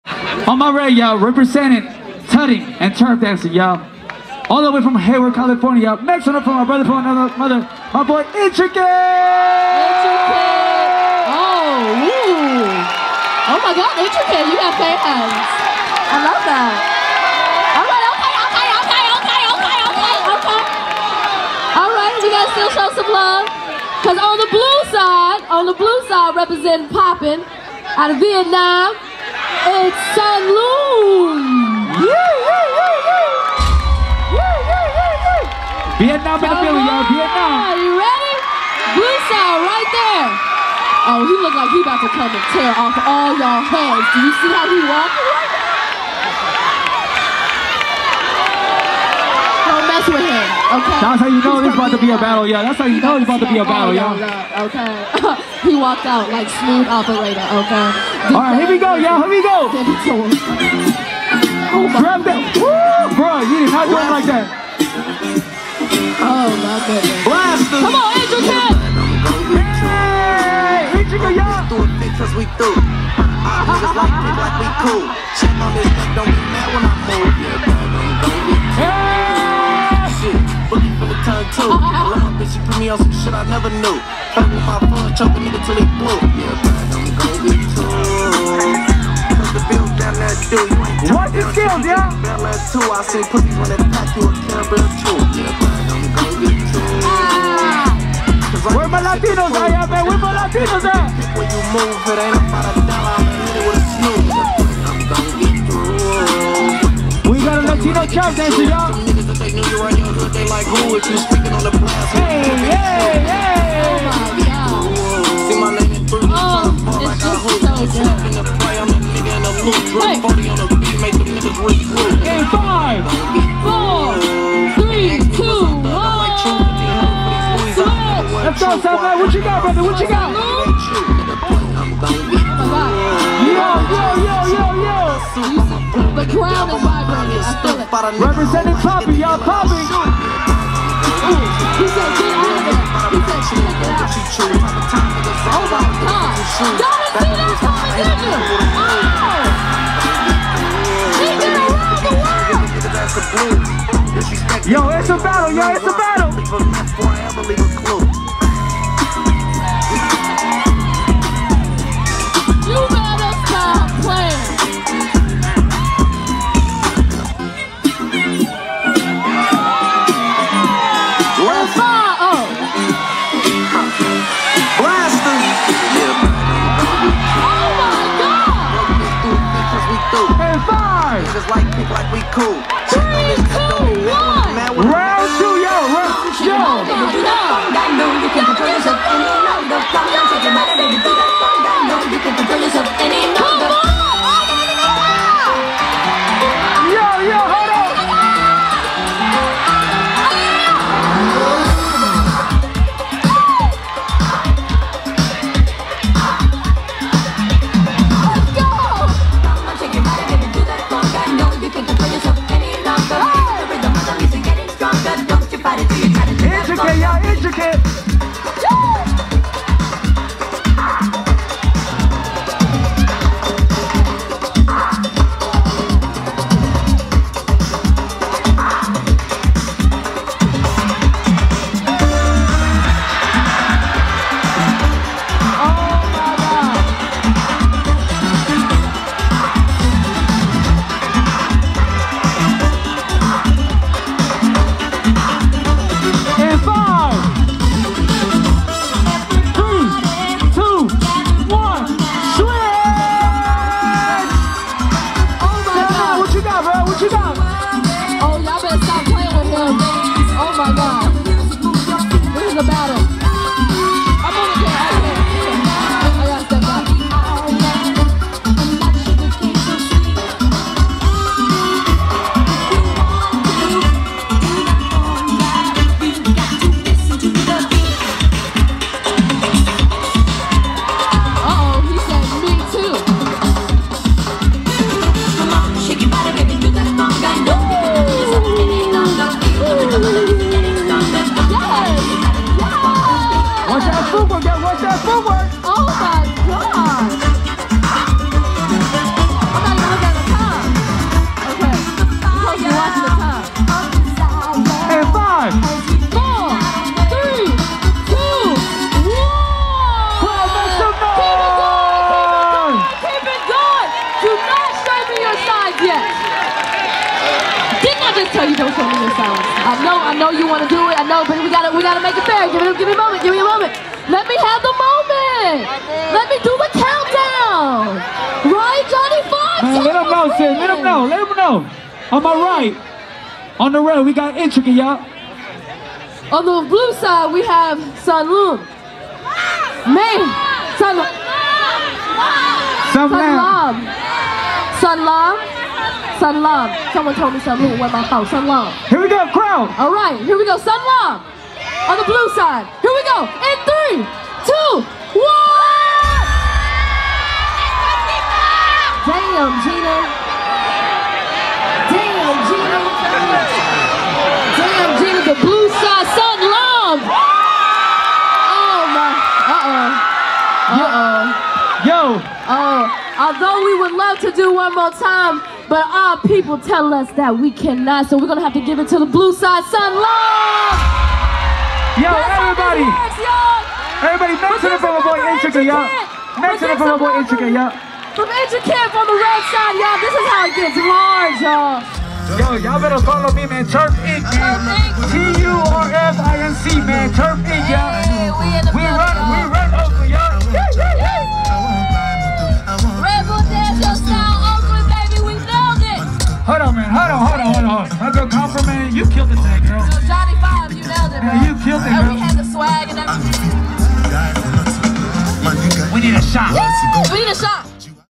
Intricate vs Sonlam Battle 7 TOP 16 TURFinc host Red Bull Dance Your Style Oakland Frank H. Ogawa
Johnny5 hosted redbulldanceyourstyle in Oakland with his partner in crime @comfortfedoke 🔥 their steez and vibe was on point! Premiered live at Frank H. Ogawa Plaza on May 6, 2022. The crowd choose the winner! @redbulldance your style Oakland edition challenges some of the best dancers in the US. From Turfin to popping to hiphop, competitors will enter epic 1-on-1 battles, bringing improvisation to a new level.

Battle 7 : Top 16
Intricate @intricacito vs Sonlam @iamsnguyen_

TURFinc 2022